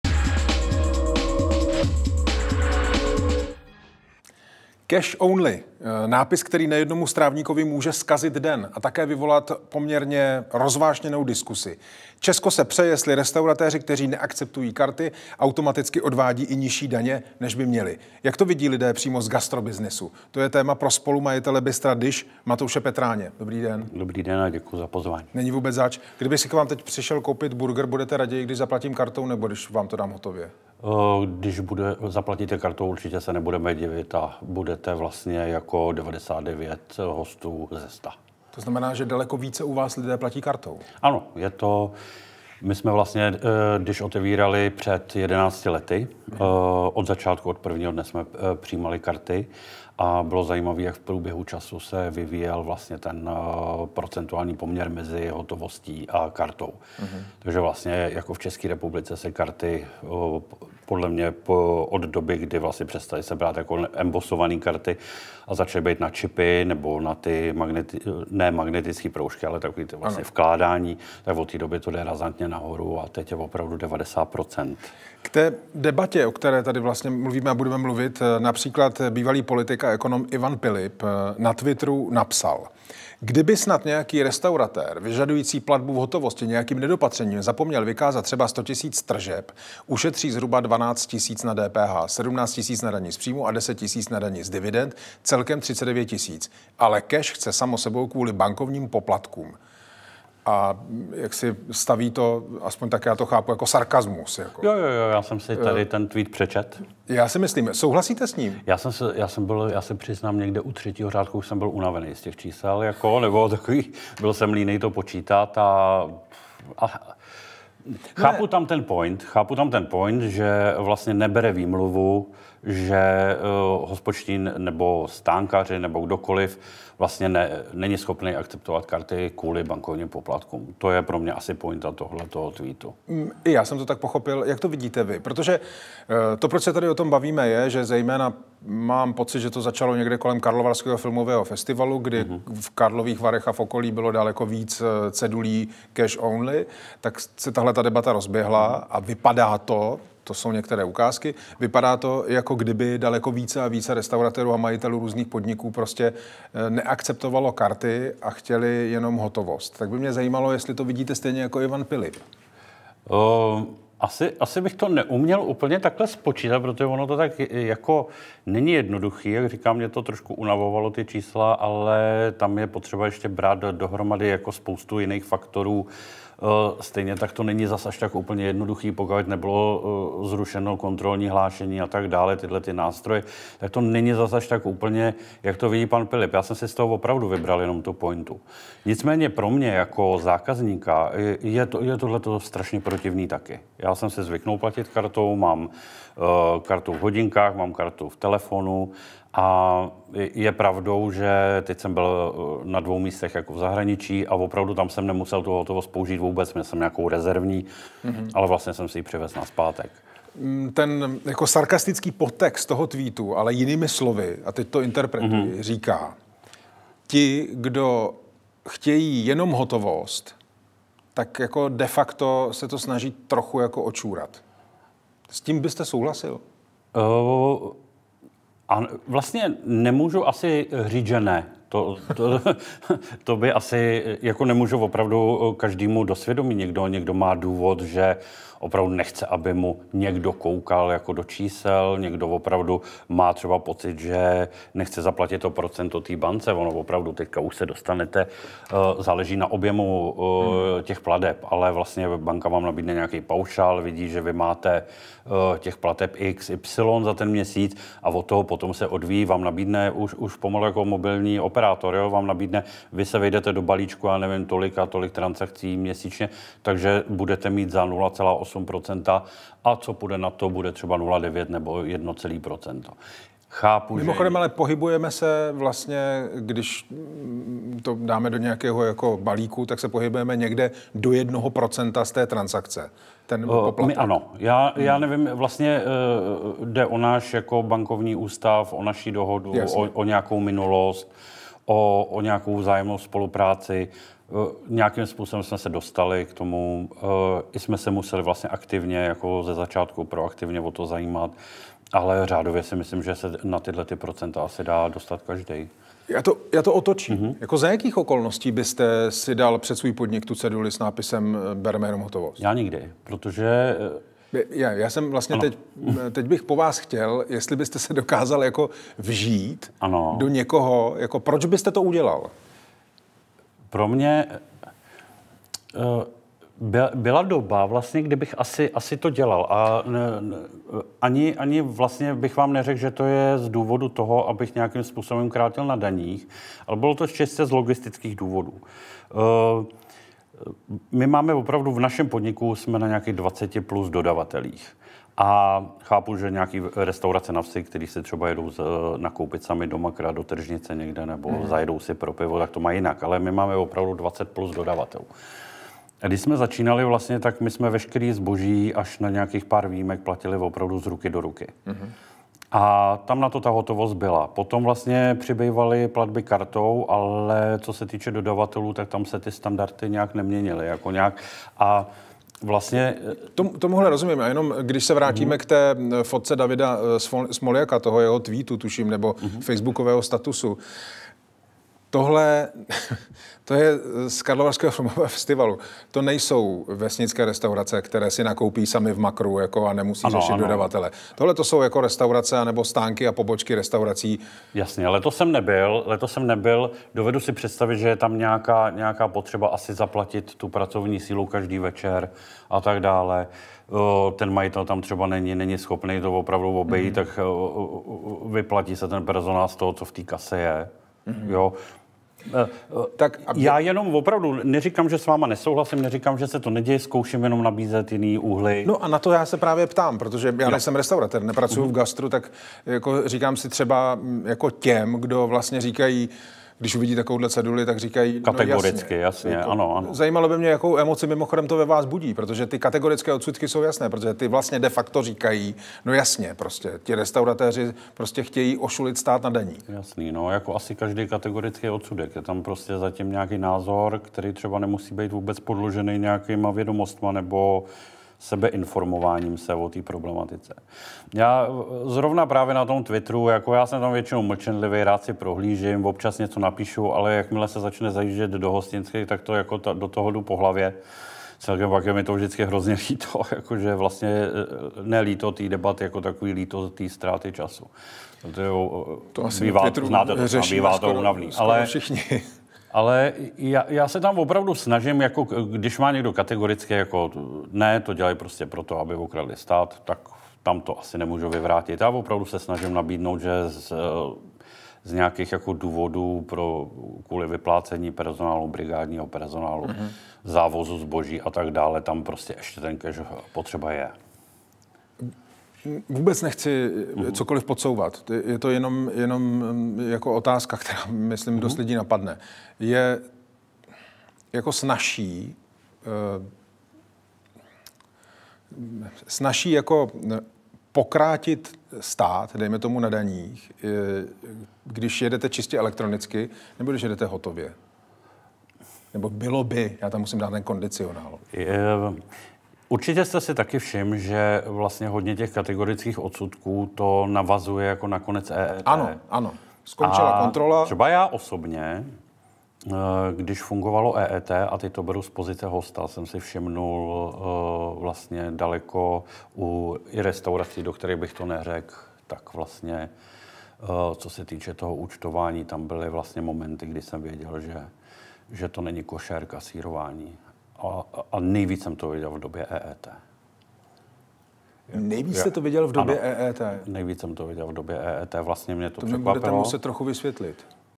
Platit v hotovosti je mi protivné, obvinění hospodských z krácení daní mě ale mrzí, říká Petráň
🔗| Celé video si pusťte ZDE: dvtv.ink/matous_petran

„Kdo chce, stát na daních pokrátí. Někdo opravdu nechce, aby mu koukali do čísel. Nejvíce kasírování, které nebylo košer, jsem viděl za EET, kdy hosté dostávali náhledové účty,“ říká spolumajitel restaurace DISH Matouš Petráň. „My karty přijímáme od začátku, snažíme se to dělat dobře a fér,“ dodává.